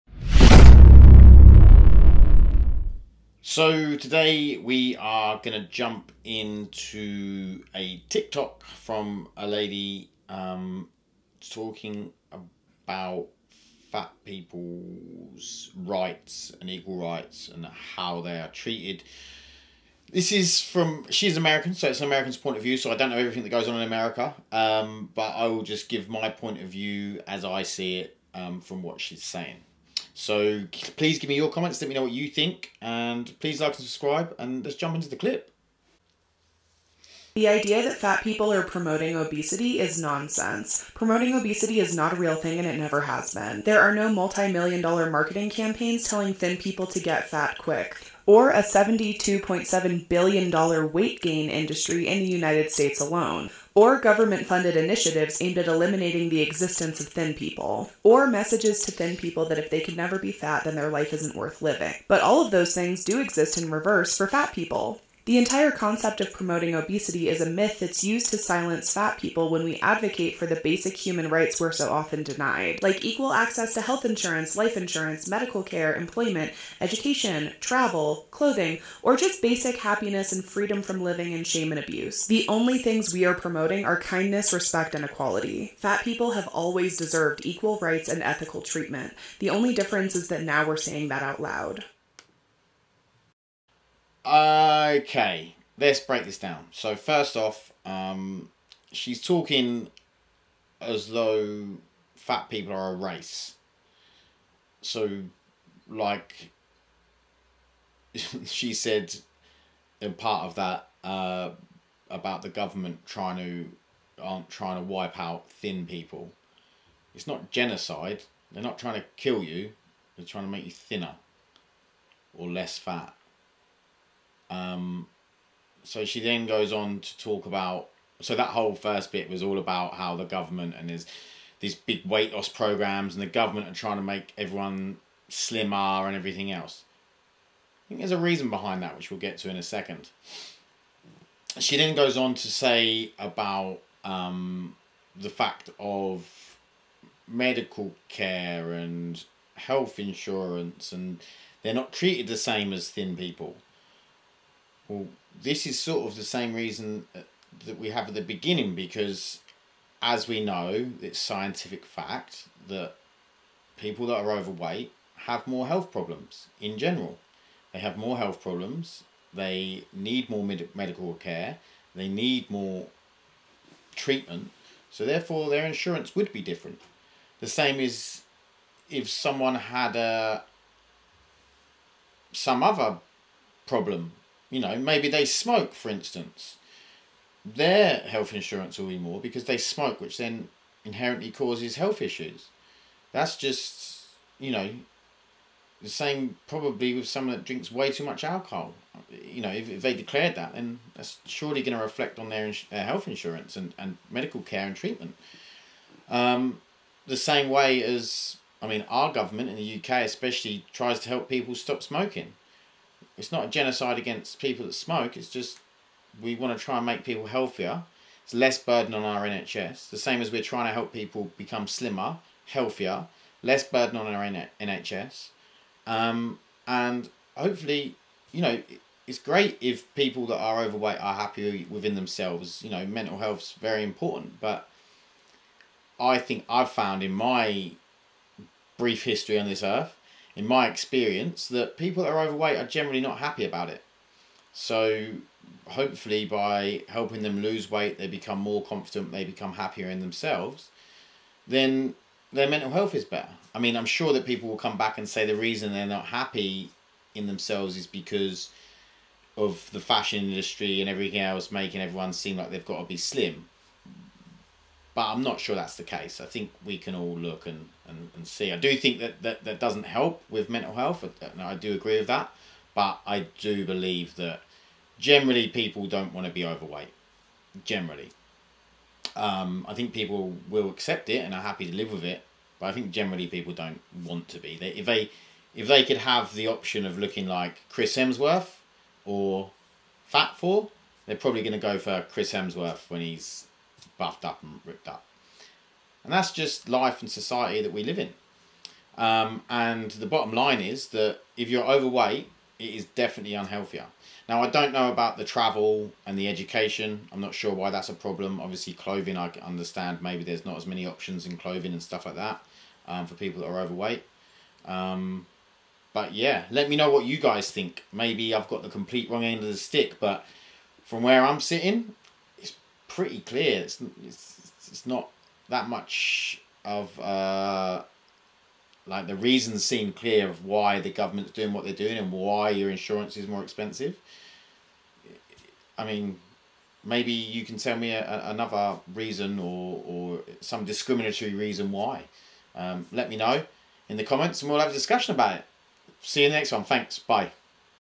Are Goverments Fatphobic? Are they trying to Eradicate Fat People?
In this video we look at a young ladies opinion on how Fat people are treated by the government and companies to do with health insurance and other areas.